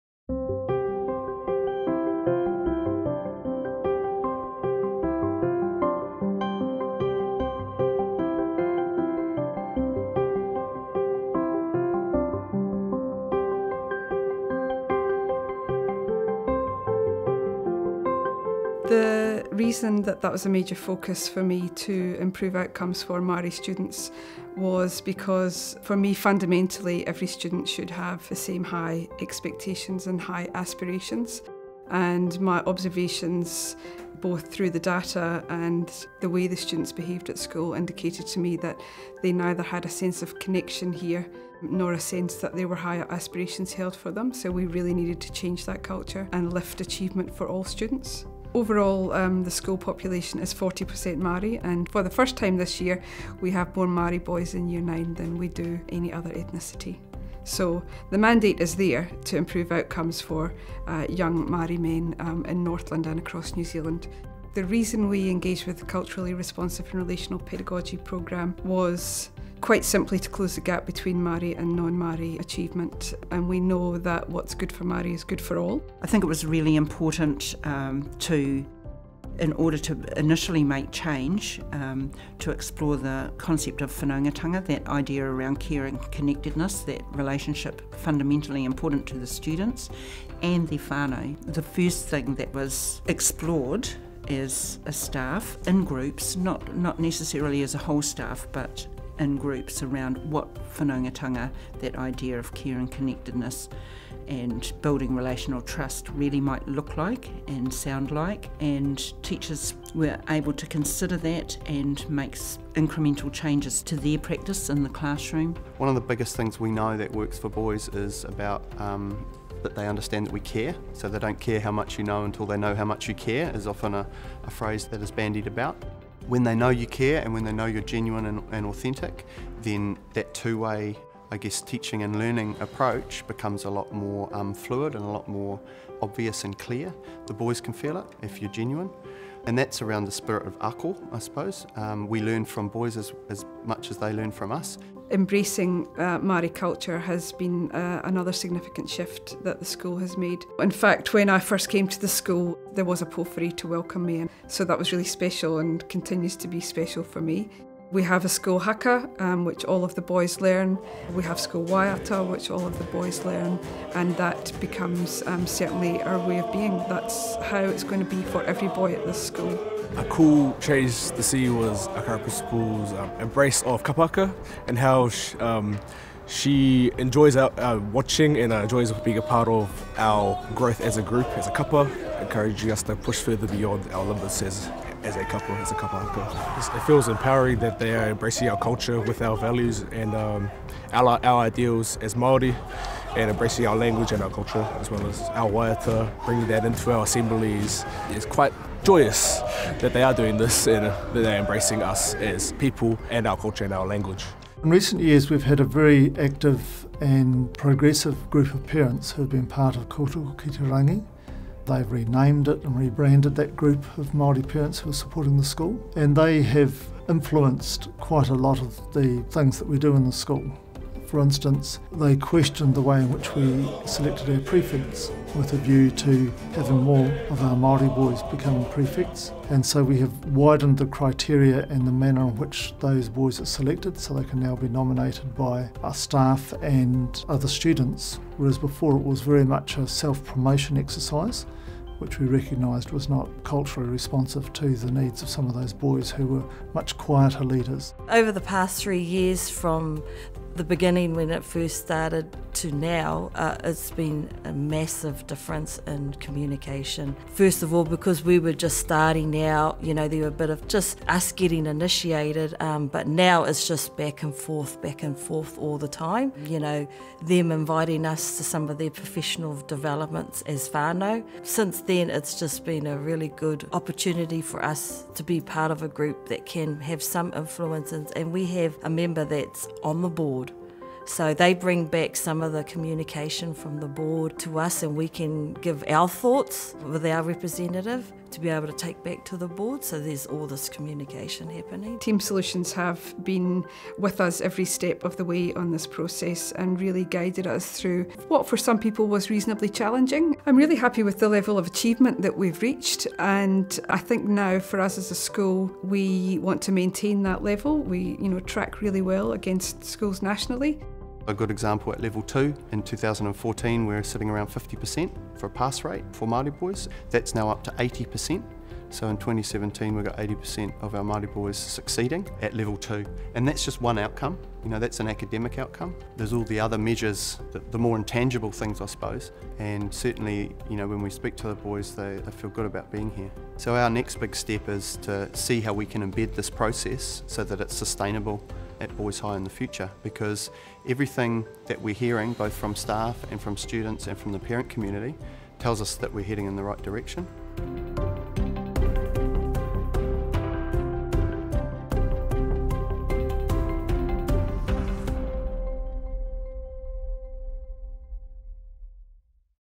Team Solutions & Whangarei Boys’ High School – Culturally Responsive and Relational Pedagogy Project
There are over 400 Māori students attending Whangarei Boys' High School. Ensuring their success as Māori is vital and is a priority for the school's leadership team. To support the school's goals, Team Solutions facilitators have worked with the school leaders and teachers to help them adapt their pedagogy to focus on a more culturally responsive and relational approach to teaching and engaging with students’ whānau, hapu and iwi.